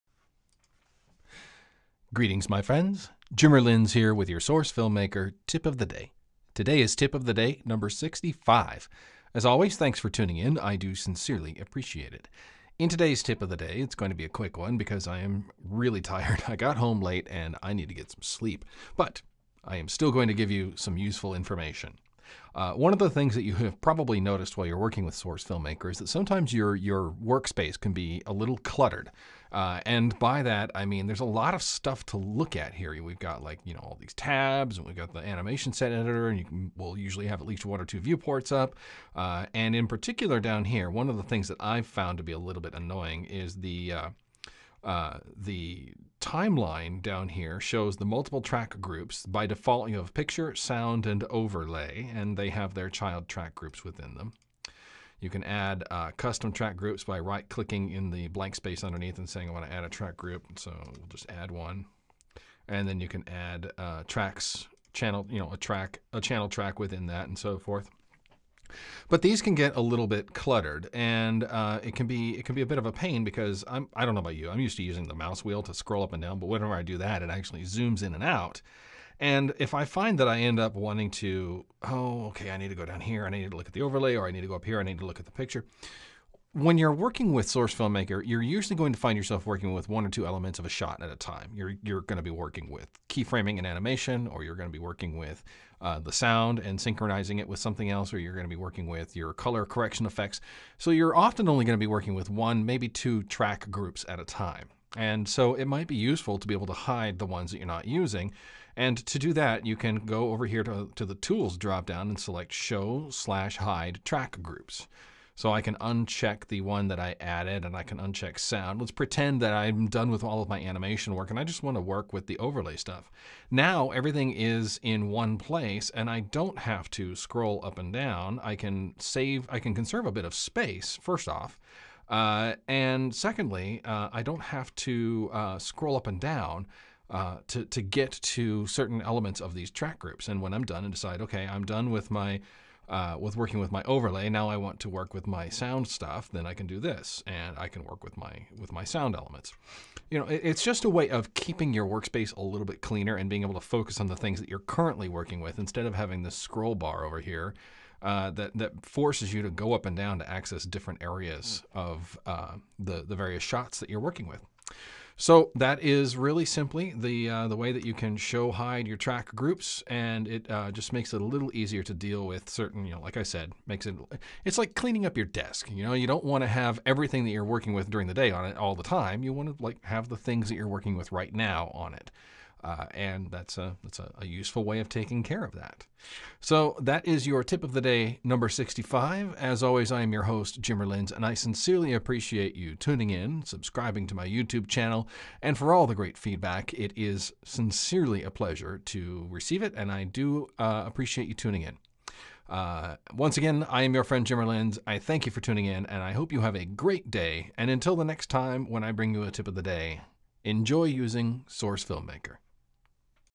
Hiding and Showing Track Groups - SFM Tip of the Day #65
In today's Tip, I show you how to show and hide Track Groups to keep your SFM workspace less cluttered.